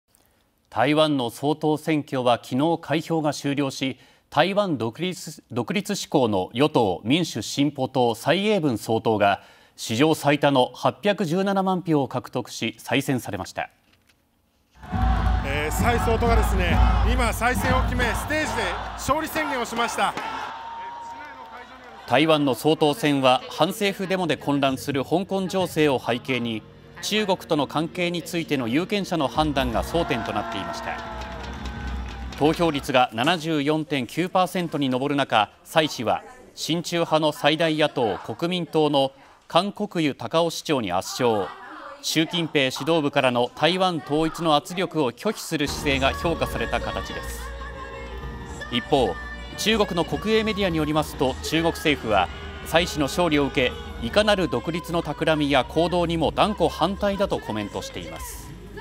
蔡総統圧勝 最多817万票
台湾の総統選挙はきのう開票が終了し、台湾独立志向の与党・民主進歩党　蔡英文総統が史上最多の817万票を獲得し再選されました。
台湾の総統選は反政府デモで混乱する香港情勢を背景に中国との関係についての有権者の判断が争点となっていました。投票率が74.9％に上る中、蔡氏は親中派の最大野党・国民党の韓国瑜高雄市長に圧勝。
習近平指導部からの台湾統一の圧力を拒否する姿勢が評価された形です。一方、中国の国営メディアによりますと、中国政府は蔡氏の勝利を受け、「いかなる独立のたくらみや行動にも断固反対だ」とコメントしています。